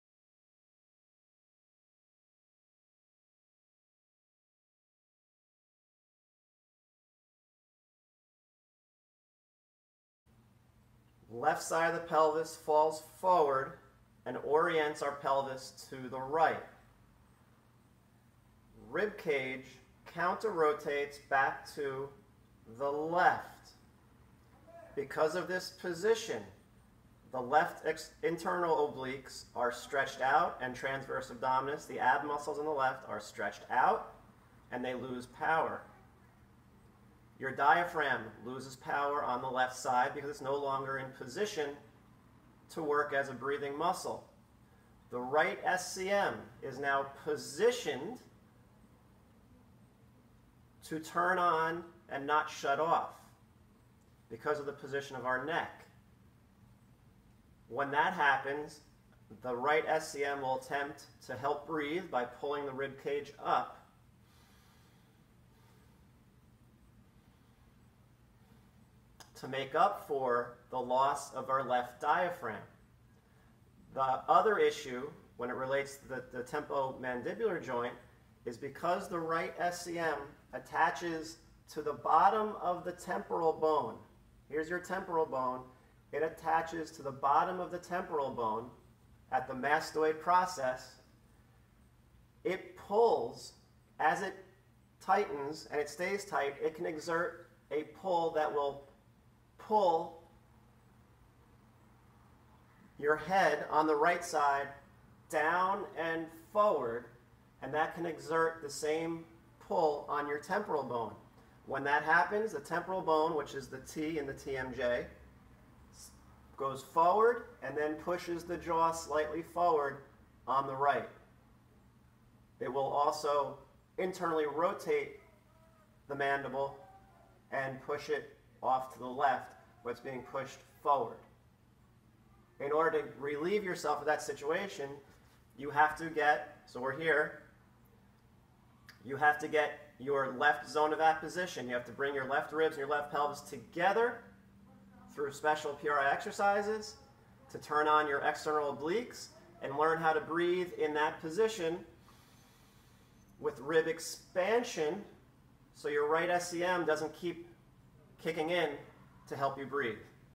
How the right SCM can influence the tempo-mandibular joint and contribute to bodily pain.
This video explains how an overactive right sternocleidomastoid (SCM) can potentially influence the tempo-mandibular joint.

In this scenario, a forwardly rotated left pelvis, accompanied by an elevated left rib cage, results in the loss of left diaphragm function.

In order to help you breathe, the right SCM will often try and help out and become over-active.

An over-active right SCM can exert a pull on the temporal bone that positions the TMJ forward on the right compared to the left and result in some type of malocclusion and restricted movement of the neck and ribcage. This can potentially contribute to posture and movement realted pain in various regions of the body.

In this scenario, the right SCM can be turned "off" by regaining proper position of the left pelvis and ribcage so that the left diaphragm regains its role as a principle breathing muscle.

This increase of left diaphragmatic function and proper position of left pelvis and left ribs will allow the right SCM to reduce its involvement in breathing, thus removing restriction of neck movements and allow proper position of the TMJ.